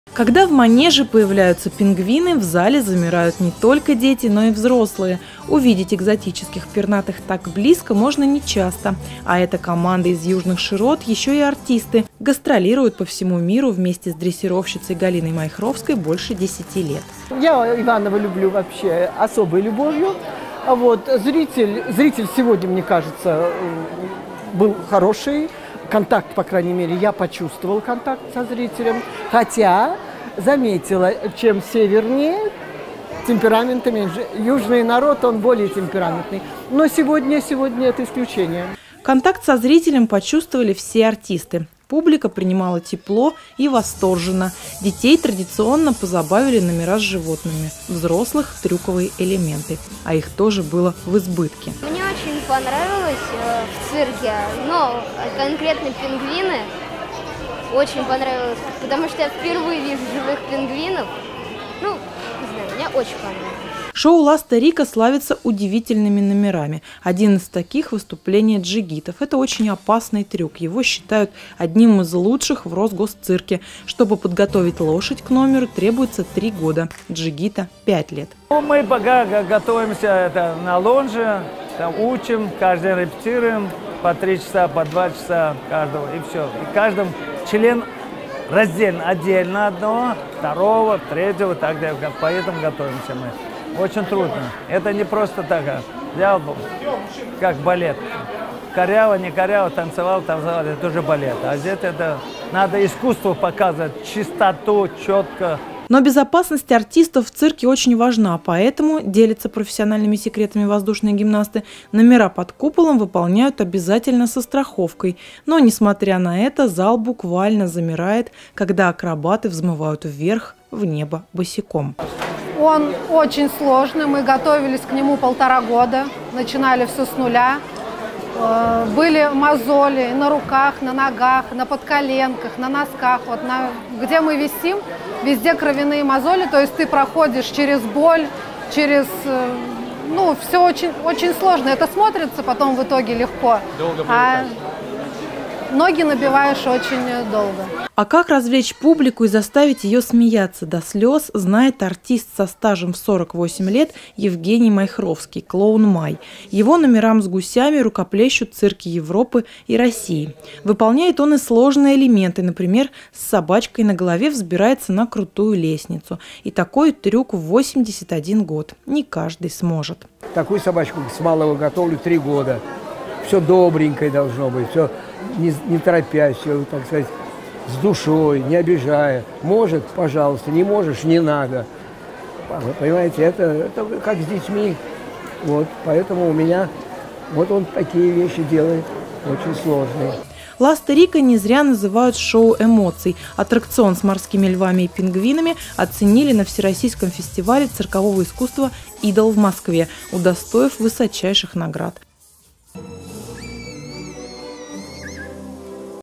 Зрители тепло приняли "Пингви-шоу Ласта Рика"
В минувшие выходные первые зрители увидели  «Пингви - шоу Ласта Рика».    Сумели ли артисты  удивить ивановцев экзотическими животные и сложными номерами – в нашем репортаже.